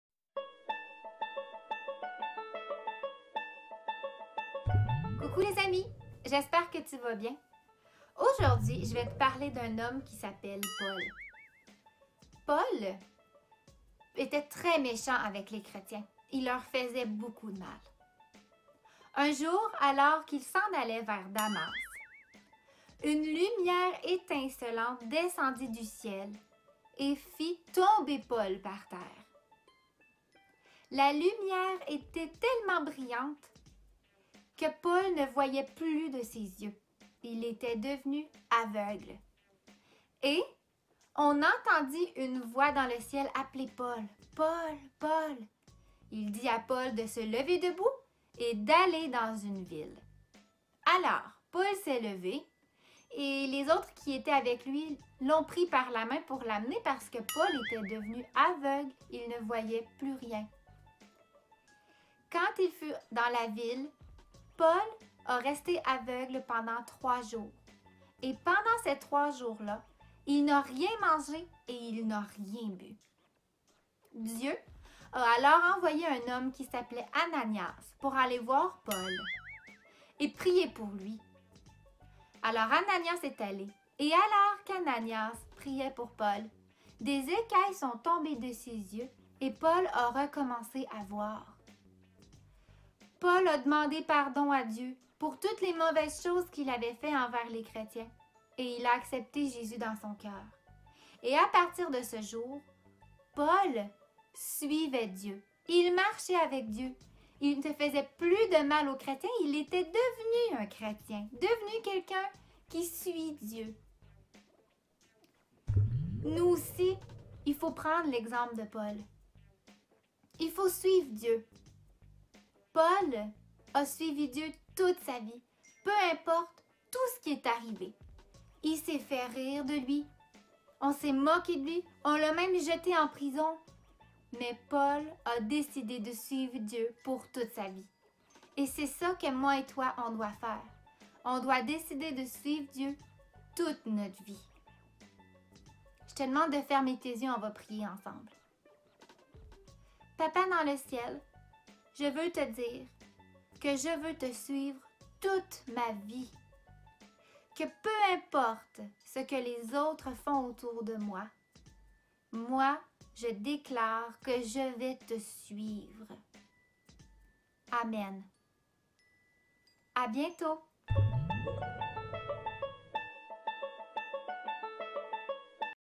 Sarah raconte la Bible - Paul
Sarah aide les enfants à suivre Jésus comme Paul